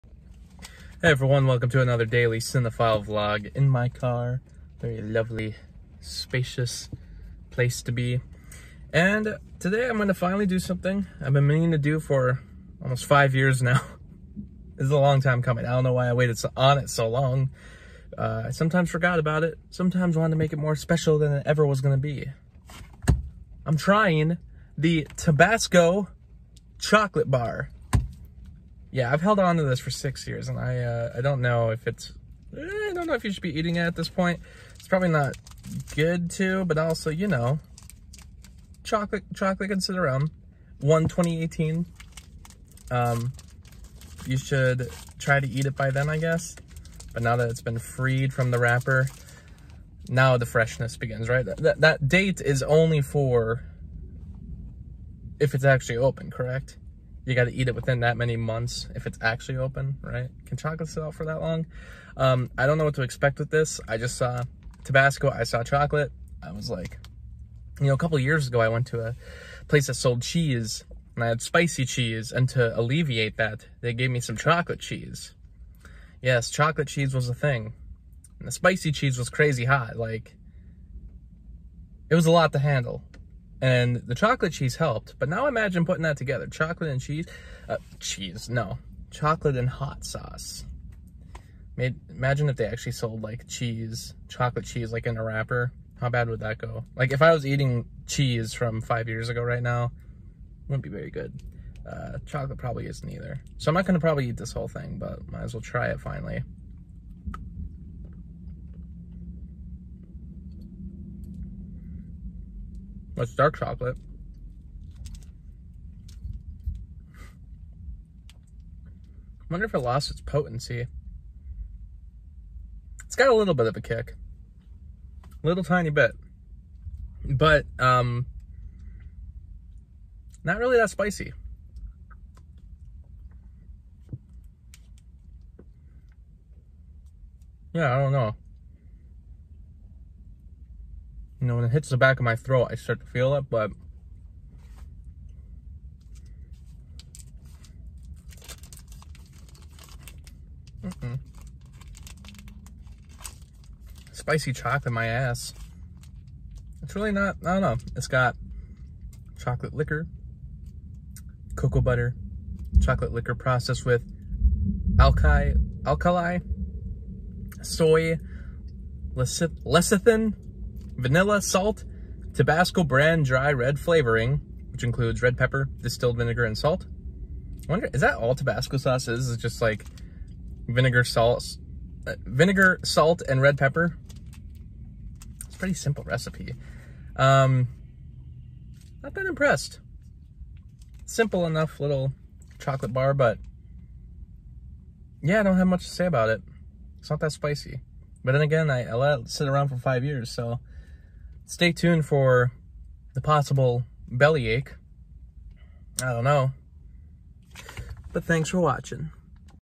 Trying a Tabasco Chocolate Bar | TDC 177
Waited 5 years for this moment